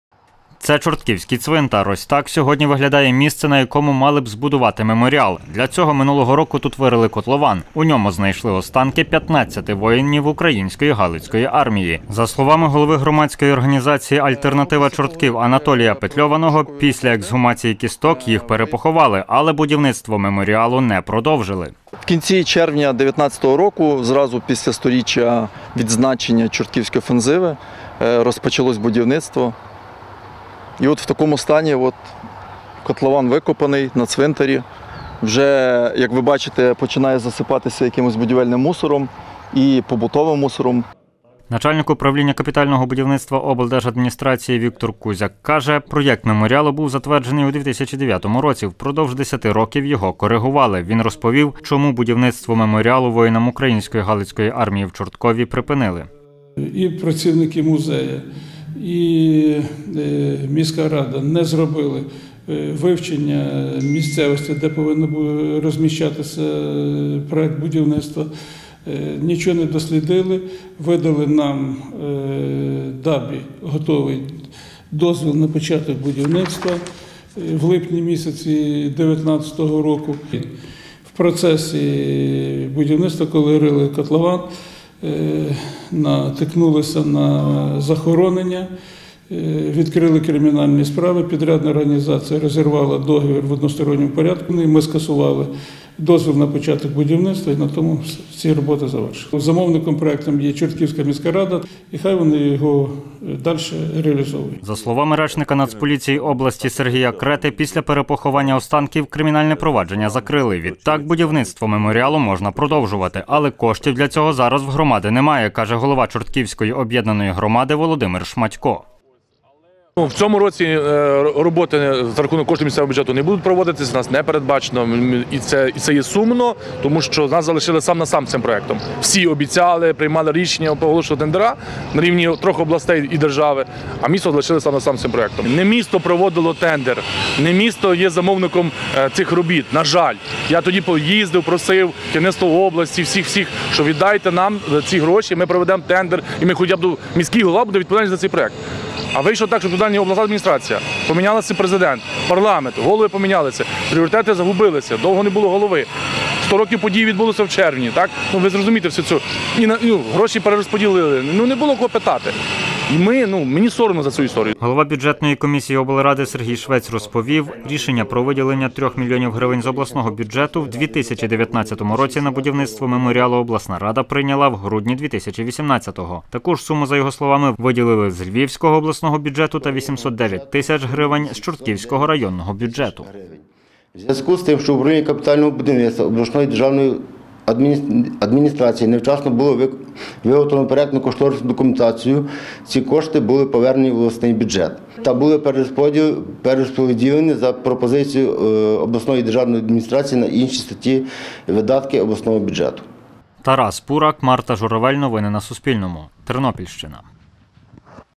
Будівництво меморіалу воякам УГА у Чорткові призупинили. Не вистачає коштів
На будівництво меморіалу воїнам Української Галицької Армії на Тернопільщині не вистачає 13 мільйонів гривень. Повідомили в Чортківській міськраді.